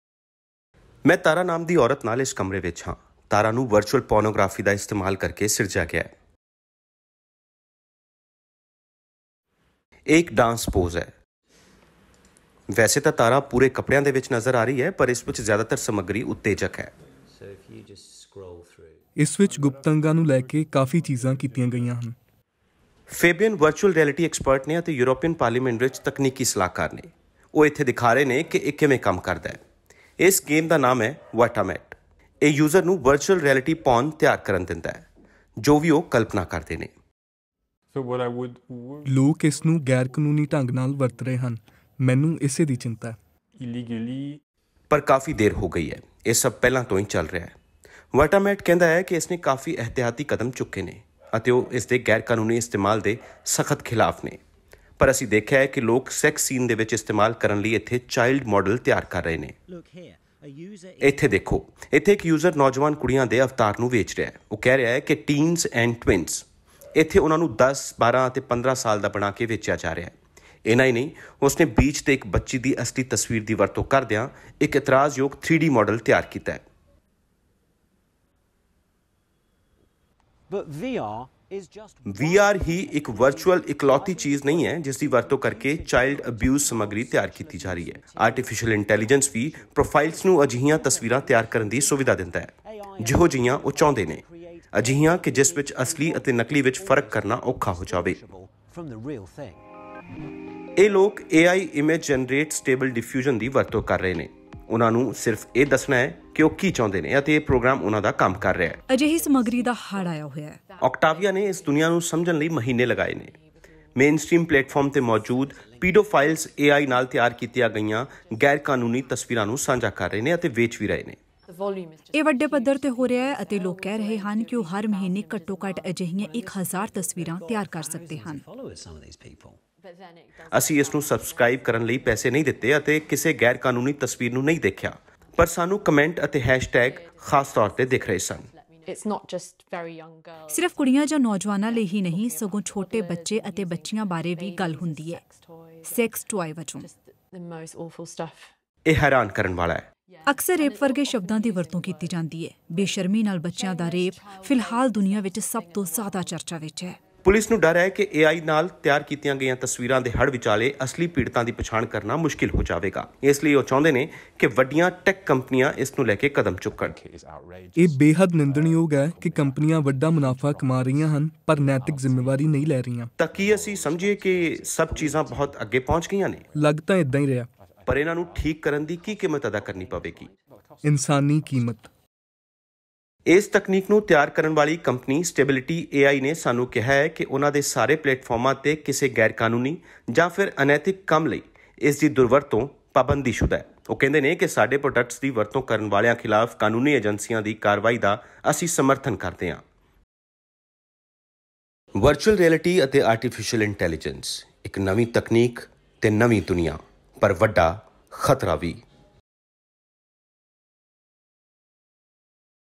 Artificial intelligence ਨਾਲ ਕਿਹੜਾ ਵੱਡਾ ਖ਼ਤਰਾ ਪੈਦਾ ਹੋਣ ਦਾ ਖਦਸ਼ਾ, ਬੀਬੀਸੀ ਦਾ ਖੁਲਾਸਾ | 𝐁𝐁𝐂
ਬੀਬੀਸੀ ਦੀ ਇੱਕ ਇਨਵੈਸਟੀਗੇਸ਼ਨ ਵਿੱਚ ਪਤਾ ਲੱਗਿਆ ਹੈ ਕਿ ਬੱਚਿਆਂ ਦਾ ਸਰੀਰਕ ਸ਼ੋਸ਼ਣ ਕਰਨ ਵਾਲੇ ਪੀਡੋਫਾਈਲਜ਼ ਆਰਟੀਫੀਸ਼ੀਅਲ ਇੰਟੈਲੀਜੈਂਸ ਅਤੇ ਵਰਚੁਅਲ ਰਿਐਲਿਟੀ ਦੀ ਵਰਤੋਂ ਕਰਕੇ ਚਾਈਲਡ ਅਬਿਊਜ਼ ਨਾਲ ਜੁੜੀ ਸਮੱਗਰੀ ਤਿਆਰ ਕਰ ਰਹੇ ਹਨ। ਮੇਨਸਟ੍ਰੀਮ ਪਲੇਟਫਾਰਮਜ਼ ਦੀ ਵਰਤੋਂ ਕਰਕੇ ਇਹ ਲੋਕ ਗੈਰਕਾਨੂੰਨੀ ਸਮੱਗਰੀ ਵੇਚਦੇ ਹਨ। ਸਬਸਕ੍ਰਾਈਬਰਜ਼ ਨੂੰ ਆਰਡਰ ਦੇਣ ਲਈ ਕਹਿੰਦੇ ਹਨ ਅਤੇ ਅਜਿਹੀਆਂ ਚੀਜ਼ਾਂ ਲਗਾਤਾਰ ਮੁਹੱਈਆ ਕਰਵਾਉਣ ਦਾ ਦਾਅਵਾ ਵੀ ਕਰਦੇ ਹਨ। ਦੇਖੋ ਐਂਗਸ ਕਰਾਊਫੋਰਡ ਦੀ ਇਹ ਰਿਪੋਰਟ।